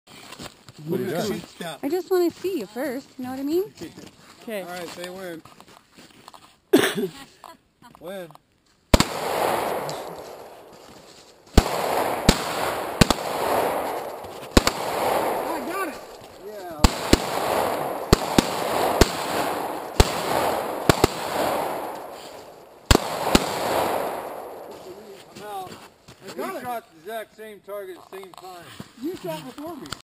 Bad Boys/Hot Fuzz Shooting Pepsi Bottle
Three men assault a Pepsi bottle.